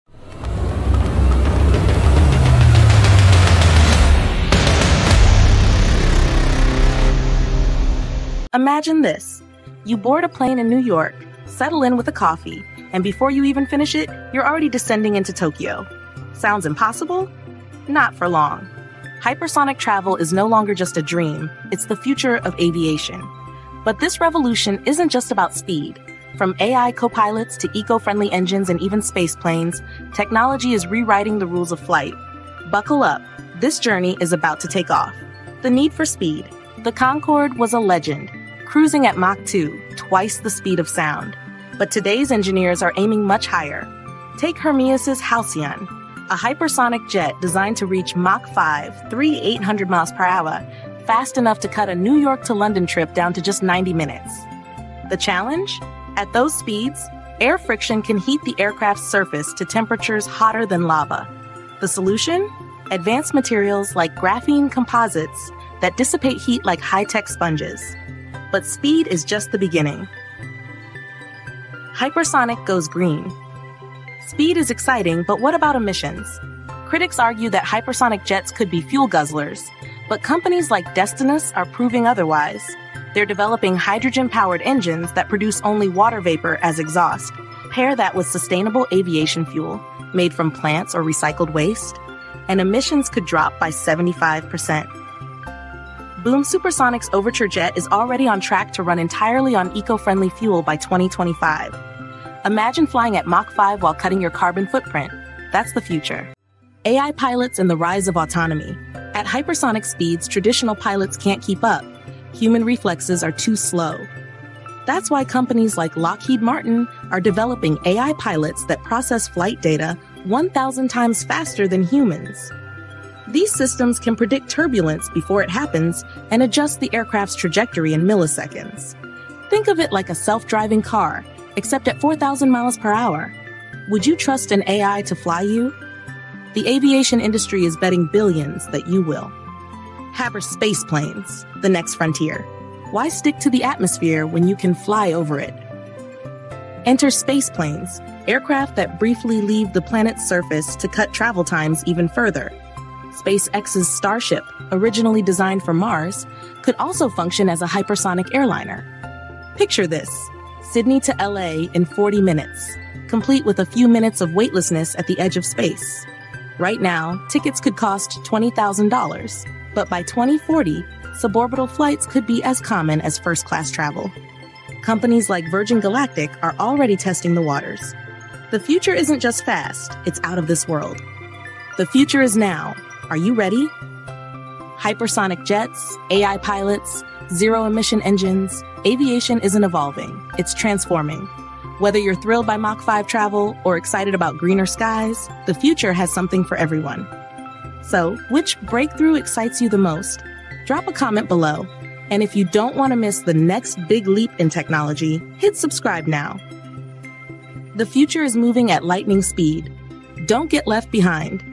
The Future of Bullet Planes Beyond Speed – Hypersonic Tech, Sustainability & Aviation Revolution
The Future of Bullet Planes Beyond Speed – Hypersonic Tech, Sustainability & Aviation Revolution @mnews5433

Are bullet planes just about speed? ✈️💨 In this video, we explore the FUTURE of hypersonic aviation—sustainability, revolutionary materials, AI pilots, and tech that could make 10-hour flights obsolete. Discover how companies like SpaceX, Hermeus, and Boom Supersonic are redefining air travel!

👉 *Chapters:*
The Need for Speed (And Beyond)
Hypersonic vs. Supersonic: What’s Next?
Eco-Friendly Bullet Planes: Myth or Reality?
The AI Pilot Revolution
Spaceplanes & Suborbital Travel

More Hashtags